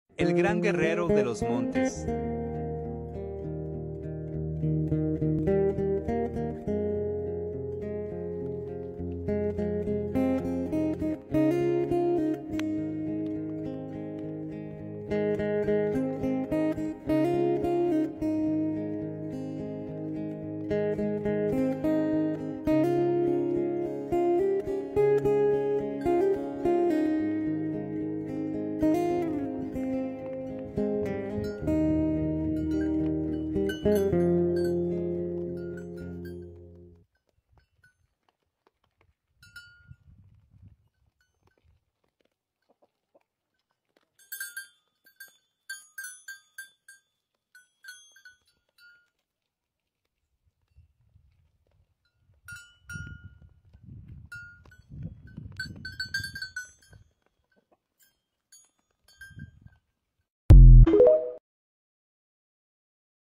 Venados de Cuitaboca
En este video se muestra como se está reservando la Fauna Silvestre en los alto de la sierra del municipio de Sinaloa